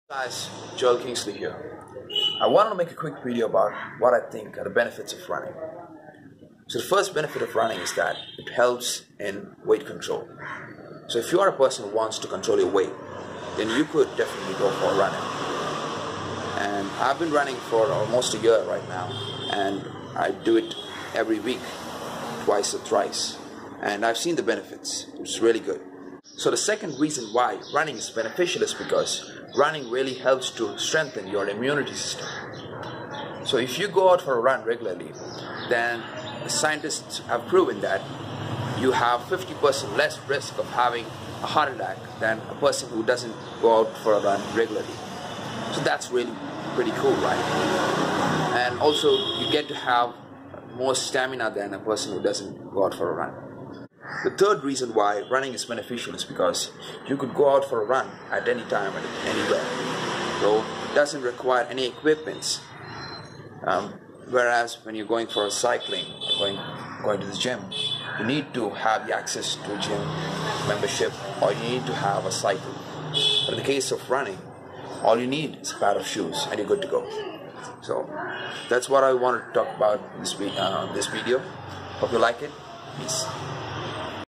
3 Benefits of Running
For someone who has been running for about a year, what do I think are the benefits of running?